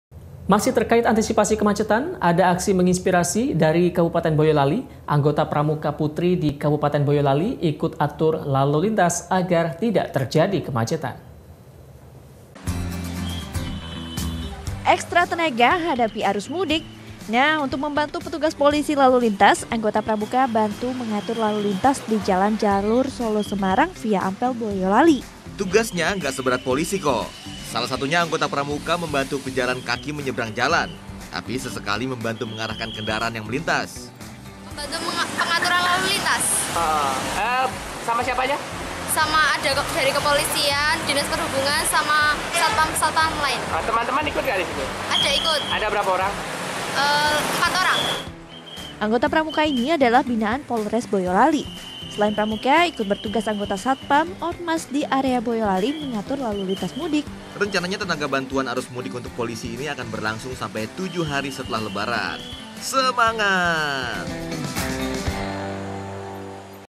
Kece, Beginilah Aksi Anggota Pramuka Putri Atur Lalu Lintas - NET JATENG
Ekstra tenaga, hadapai arus mudik. Nah, untuk membantu petugas polisi lalu lintas, anggota pramuka bantu mengatur lalu lintas di lintas jalur Solo-Semarang, via Ampel Boyolali.

Tugasnya, ga seberat polisi kok. Salah satunya anggota pramuka membantu pejalan kaki menyeberang jalan. Tapi sesekali membantu mengarahkan kendaraan yang melintas.

"ia baru pertama kalinya saat arus mudik seperti ini ikut mengatur dan menyebrangkan warga yang melintas, ia ditugaskan bersama tiga orang anggota pramuka putri lainnya di posko Ampel ini, ia sangat senang sekali,” ujar LIA – Siswi SMKN 1 Boyolali.

Anggota pramuka ini adalah binaan polres boyolali.selain pramuka, ikut bertugas anggota satpam, ormas, di area Boyolali, mengatur lalu lintas mudik.

Rencananya tenaga bantuan arus mudik untuk polisi ini akan berlangsung sampai 7 hari setelah lebaran.